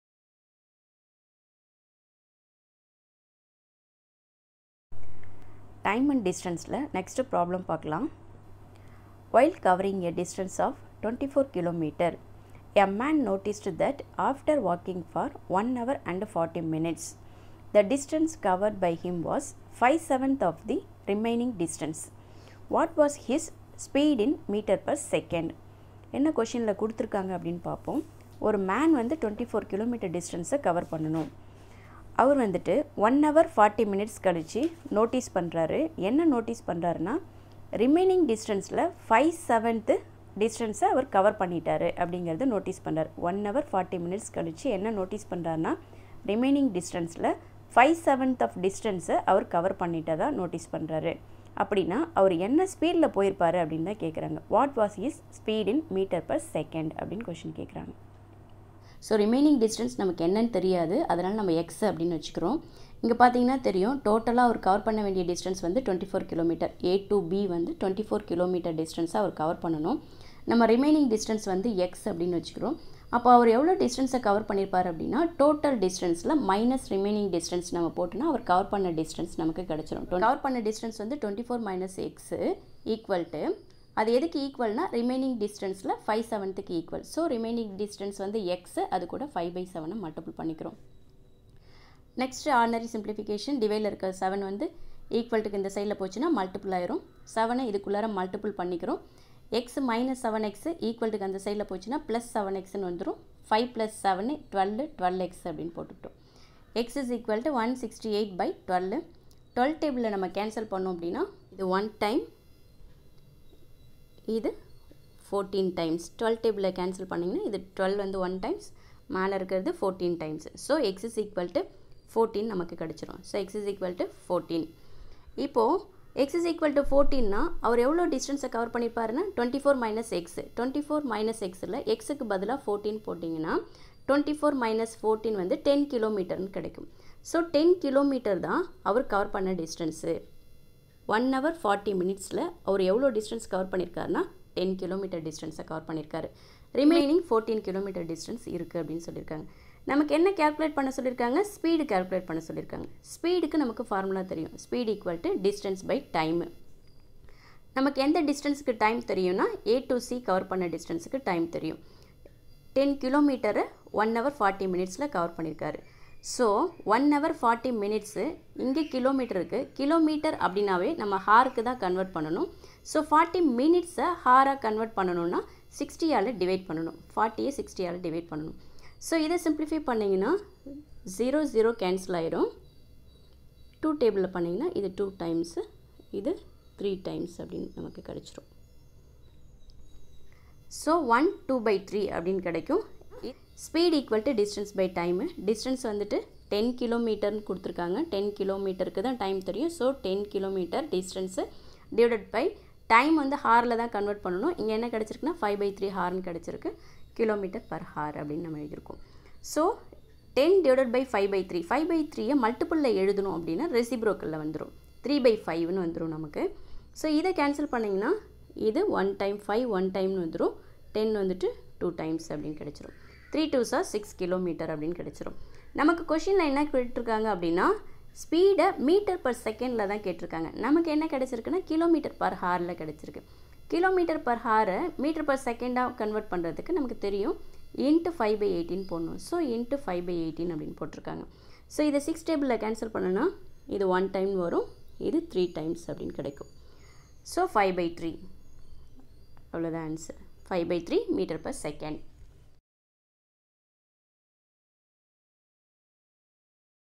While covering a distance of 24 km, a man noticed that after walking for 1 hour and 40 minutes,the..
Time and distance related problems in Tamil